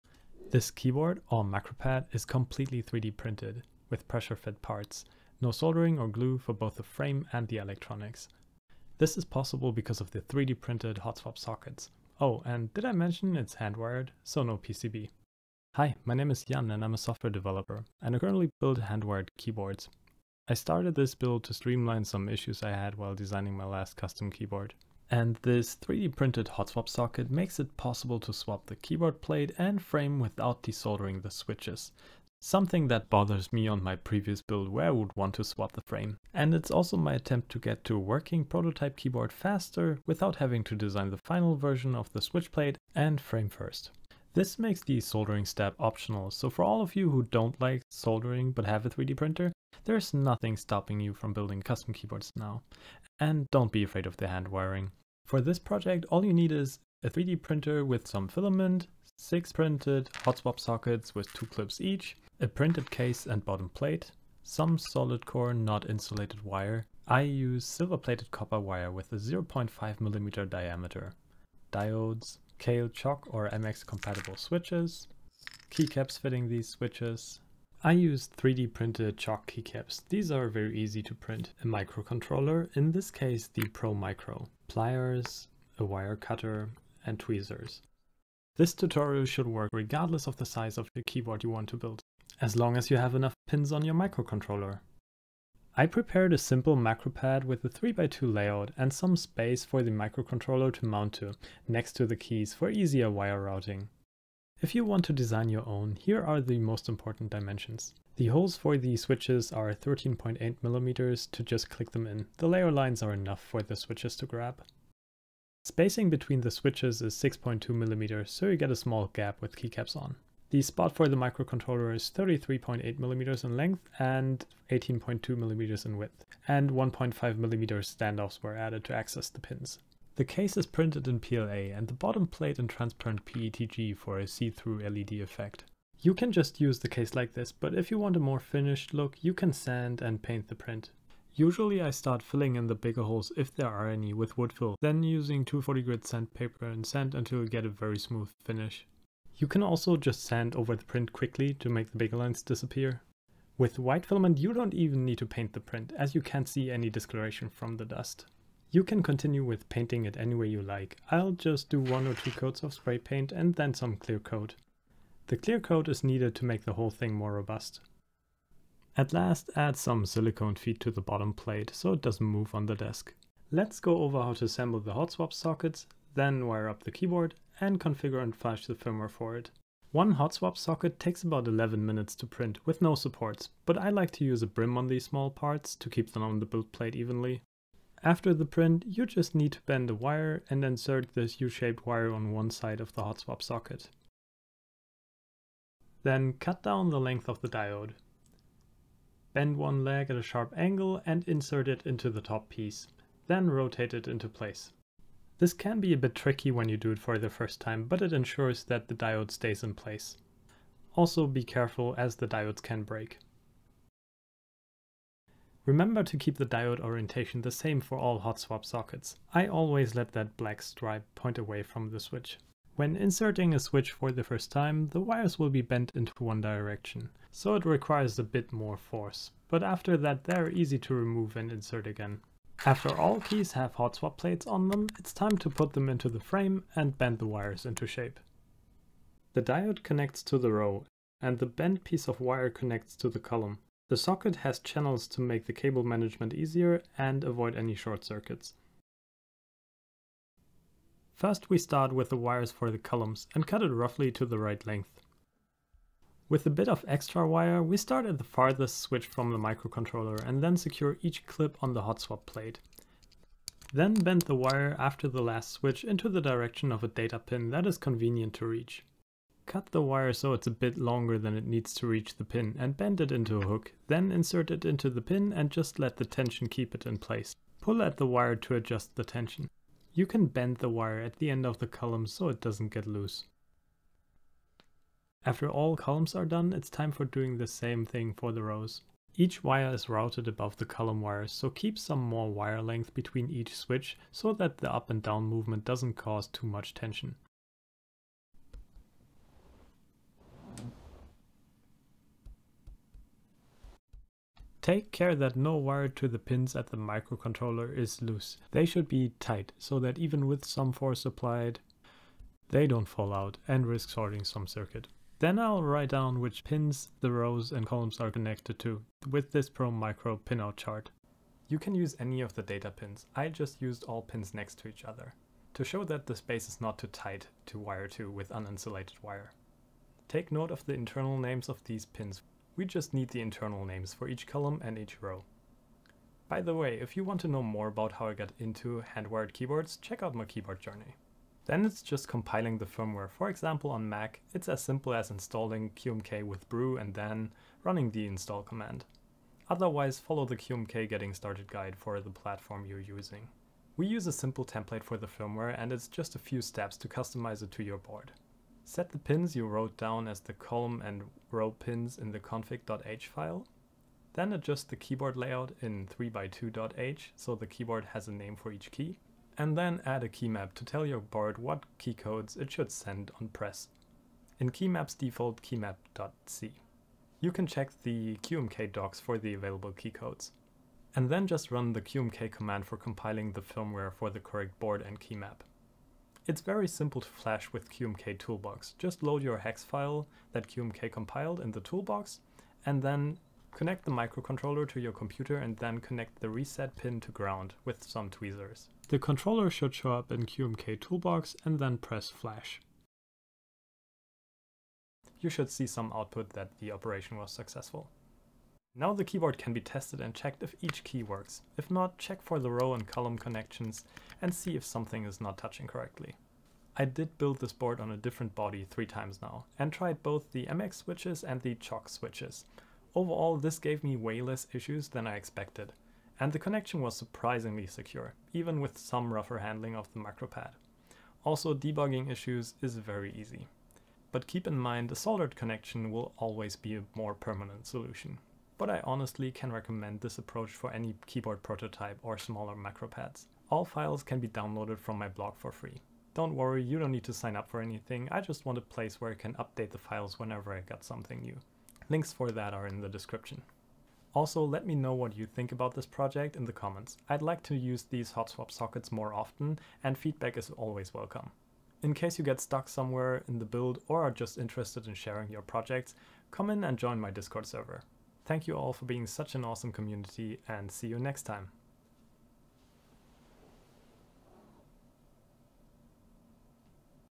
No Soldering and no PCB Macropad build | macro pad part 1
this 3by2 mechanical macropad supports mx or choc switches with my 3d printed hotswap plates with the goal to build a prototype faster and have no soldering required.

UPDATE: the firmware is now in my git repository so you all receive updates
and the 3d print files are in a separate repo as well

Materials:
Diodes 1N4148

0:00 Intro
0:55 Materials
1:28 Case Design
3:00 Hotswapsocket
3:55 Wiring
5:15 ProMicro Pinout
5:41 Firmware
6:57 Review